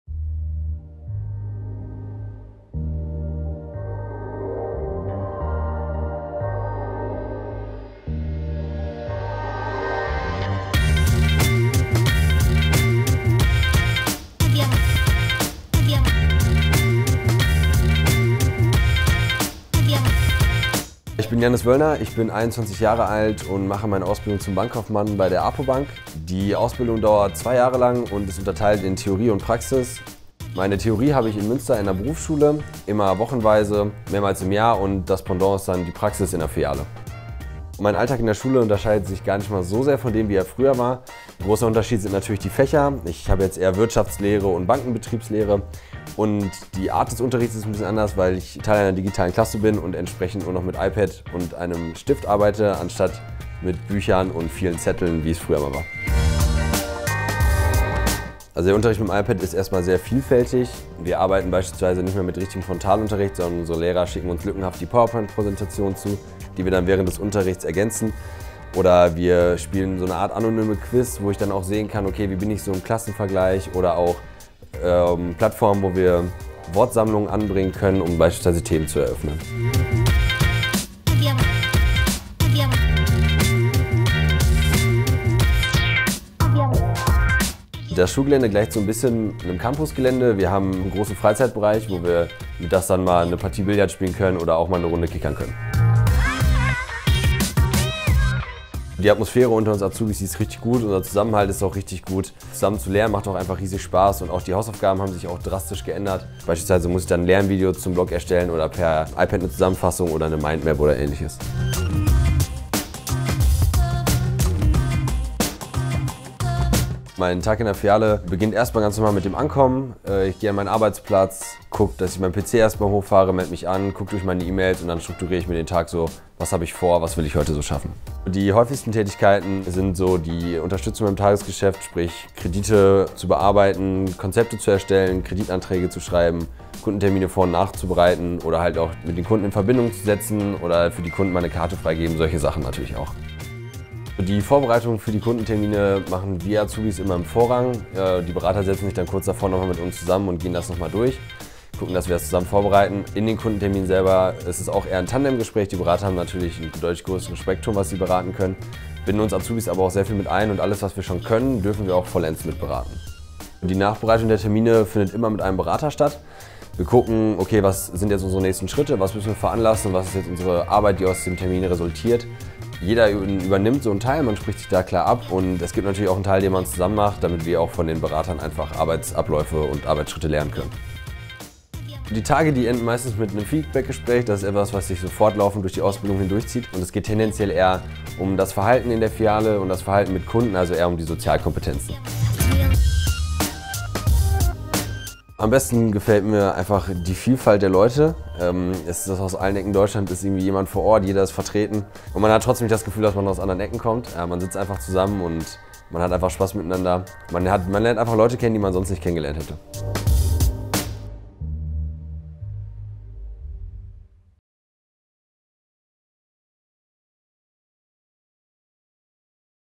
Ausbildung zum Bankkaufmann bei der apoBank - One day in a Job
Blicken Sie ein in die Ausbildung zur Bankkauffrau oder zum Bankkaufmann bei der apoBank. Unser Auszubildender Jannis Wöllner nimmt Sie einen Tag lang mit in seine Ausbildung zum Bankkaufmann.